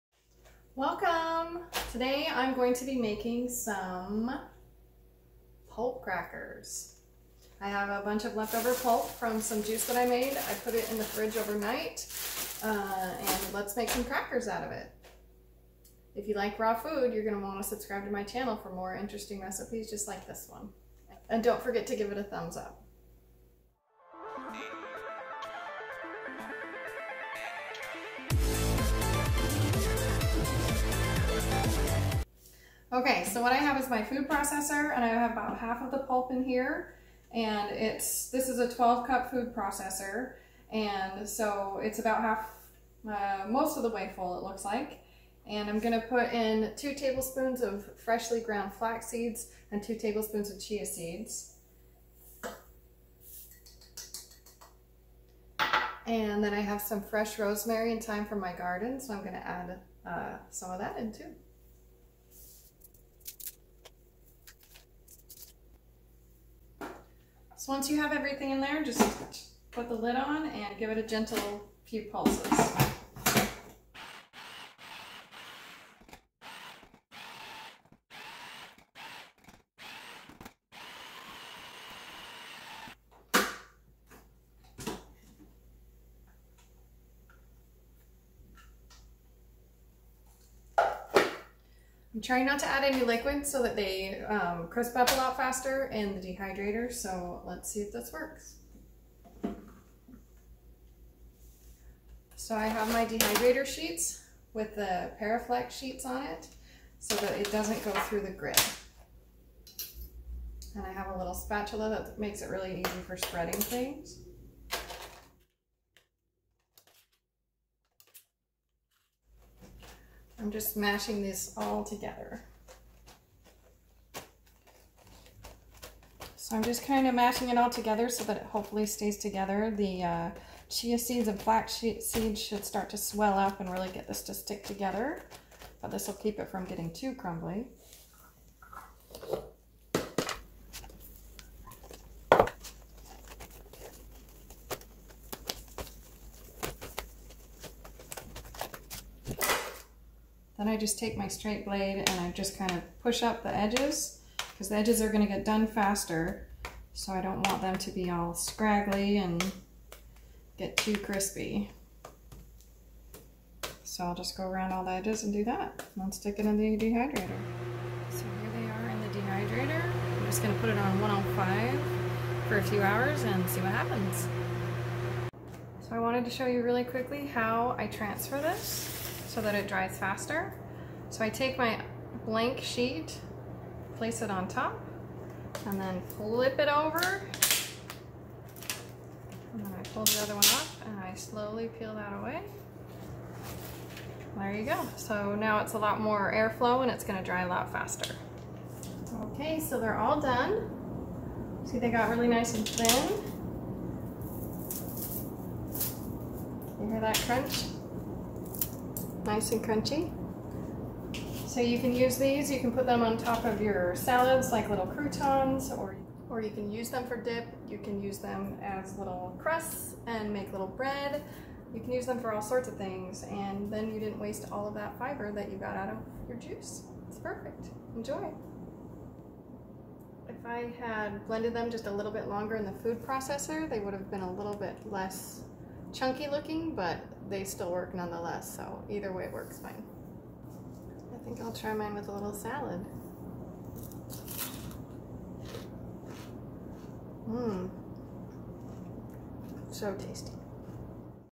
Raw Vegan Pulp Crackers With Pulp From my Juicer
Raw vegan pulp crackers from my juicer pulp! This was a juice made of beets, carrots, oranges, strawberries and romaine lettuce. I added rosemary and thyme to the pulp and 2 TBS flax seeds and 2 TBS chia seeds. Dehydrate at 105 degrees for about 8 hours or until as crisp as you like!